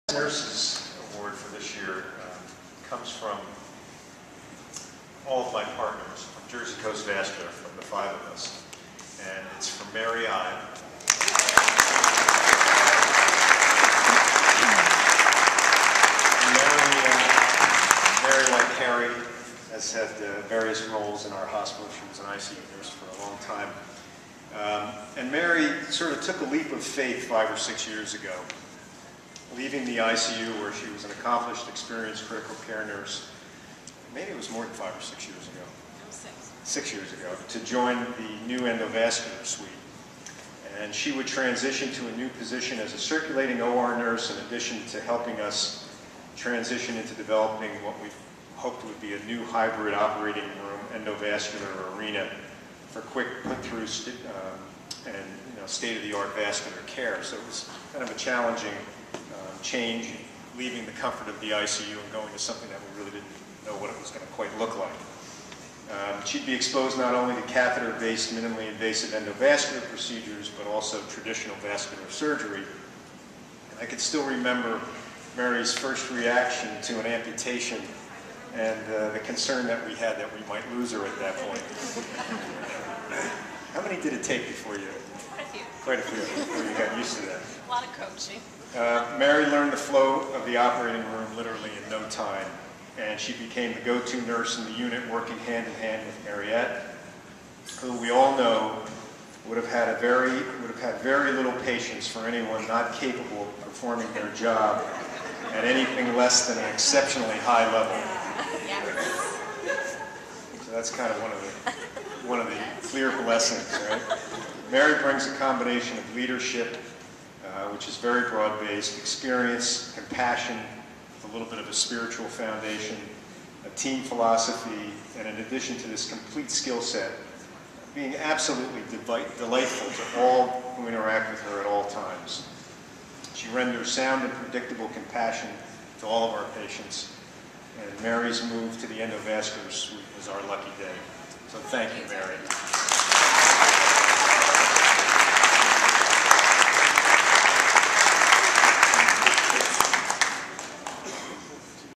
Physician's Choice Nurses of the Year: Mary Eib
Physicians at Ocean Medical Center took time during National Doctor's Day to honor nurses who are valuable and respected partners in caring for patients. Dr. Frank Sharp and his colleagues at Jersey Coast Vascular Institute recognized nurse Mary Eib for her work in the Endovascular operating room.